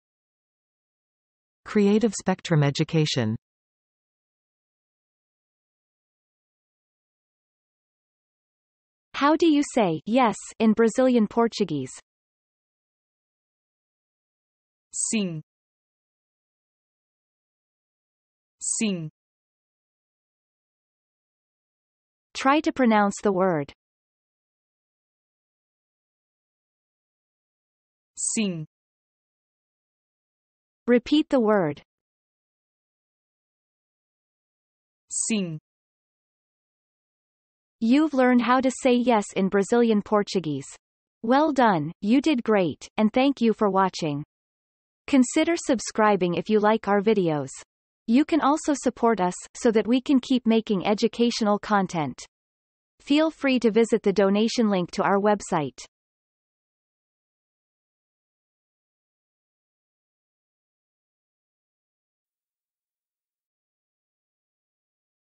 How do you say "yes" in Brazilian Portuguese? | How to say "yes" in Brazilian Portuguese?
This Brazilian Portuguese vocabulary lesson for English speakers is provided to you by Creative Spectrum Education.